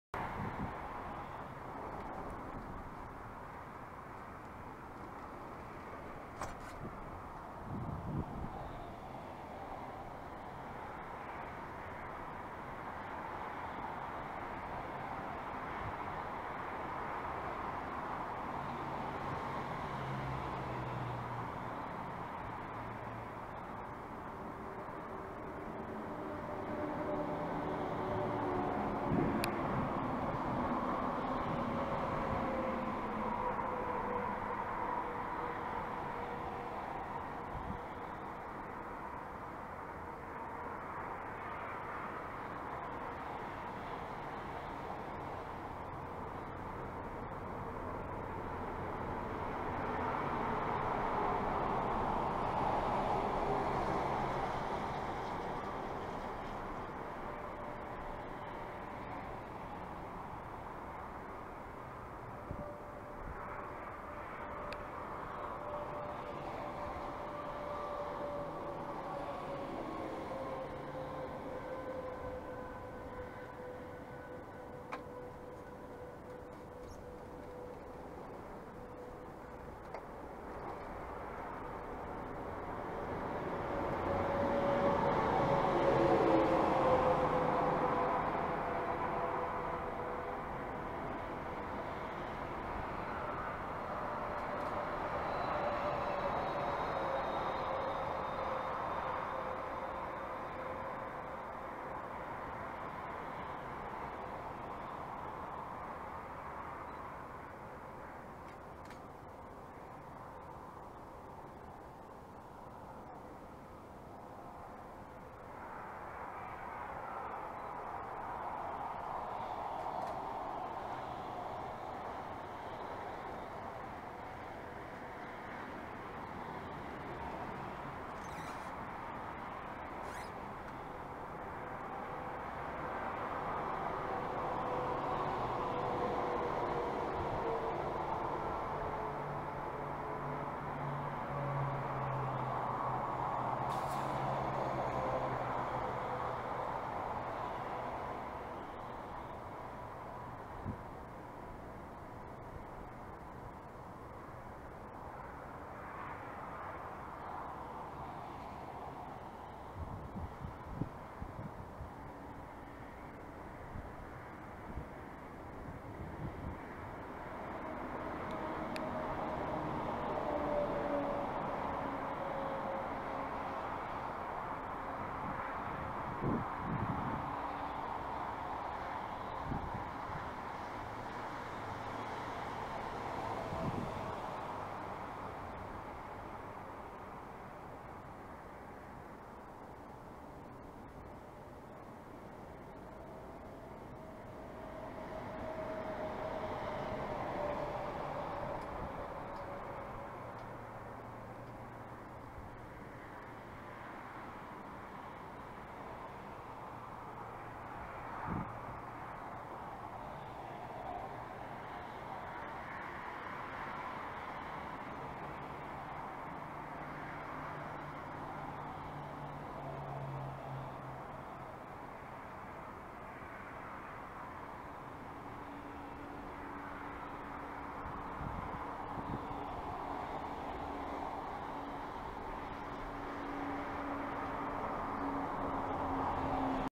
Sunrise - February 14, 2015
Not a whole lot of sun but some really nice colors. Shot with my Midland XTC 280.